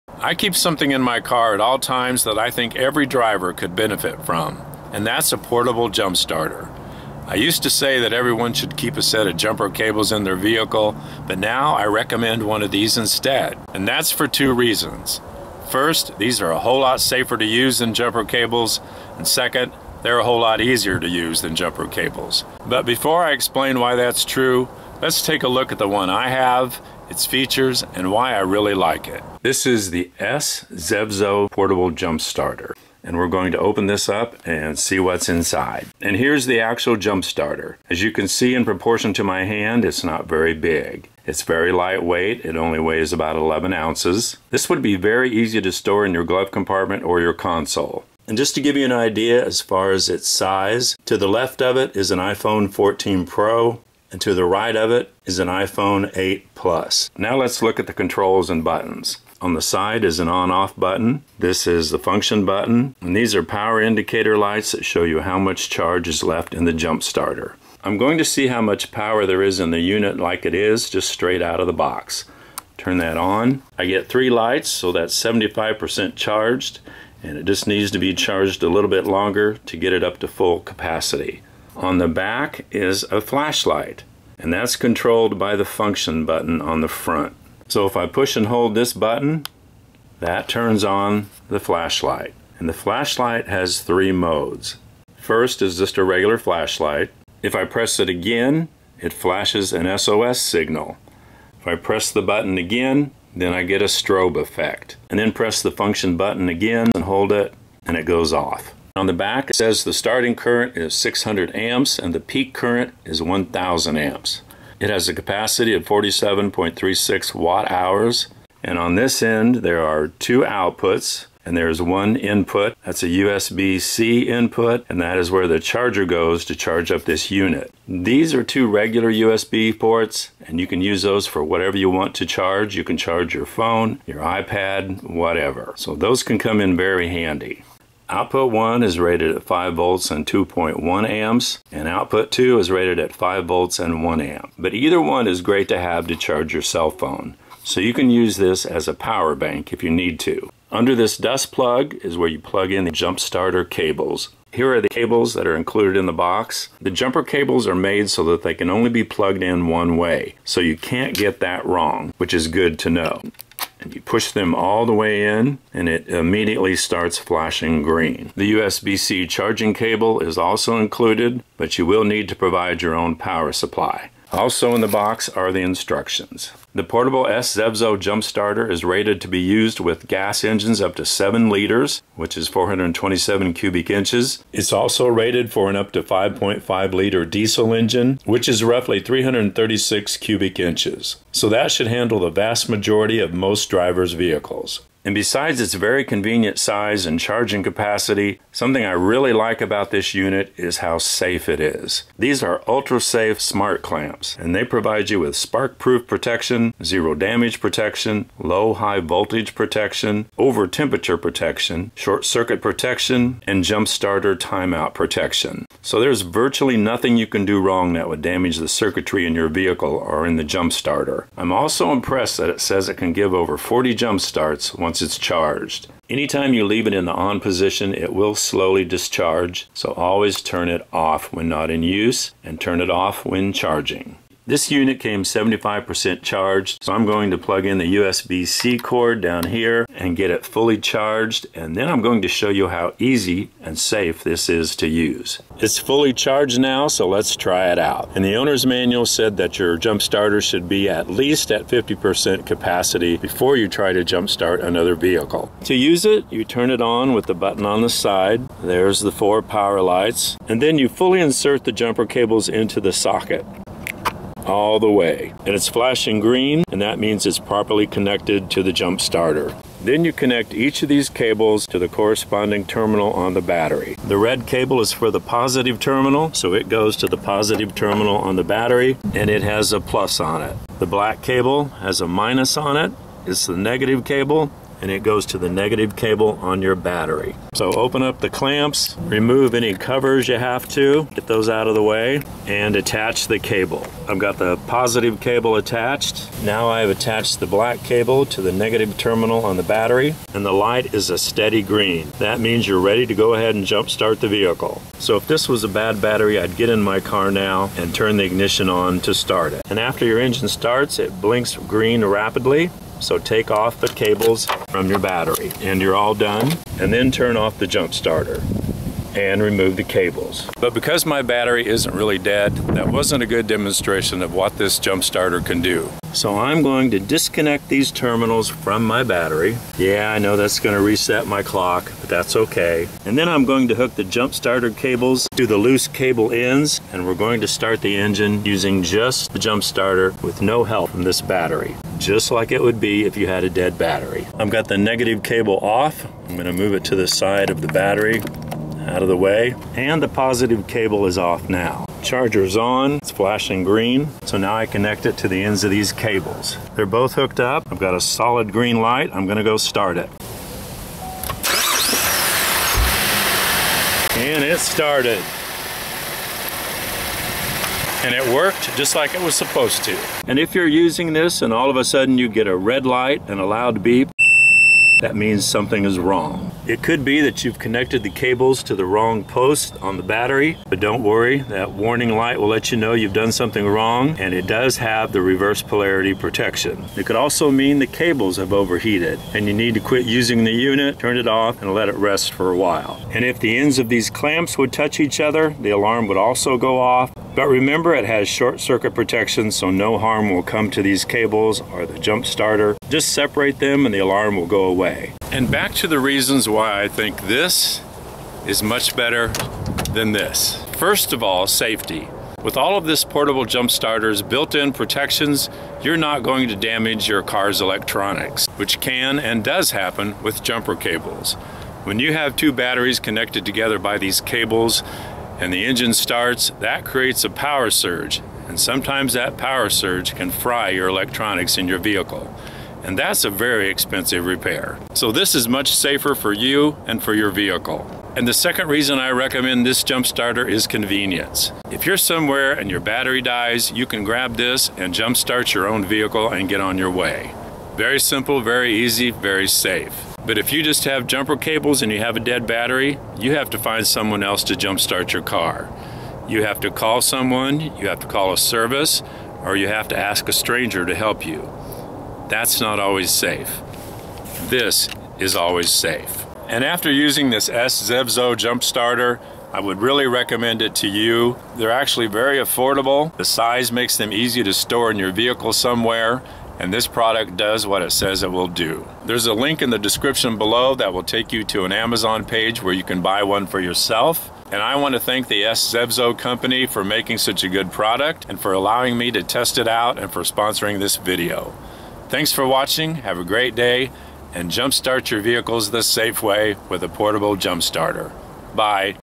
The S ZEVZO 1000A Is Why You Need To Get A Portable Jump Starter & Get Rid Of Your Jumper Cables!
This deal is valid from March 11 - March 31, 2023!

I keep something in my vehicle at all times that I wish everyone had in theirs, and that's a portable jump starter! Mine has come in handy several times, done its job, and saved me a lot of time and inconvenience. It might have even saved me money by my not having to call a roadside service to help jump start my car. But what it has taught me for sure is that a portable jump starter is much safer and much easier to use than jumper cables...I never want to have to use jumper cables again! I'm totally sold on them, and I think you would benefit from having one as well. The jump starter in this video is from S ZEVZO, and it is an excellent one. It's very compact, easy to use, and it works like a charm! I want to thank them for sponsoring this video, and providing the jump starter for me to review...it's a great product. I highly recommend you get one for yourself by clicking the link above, and put your jumper cables away!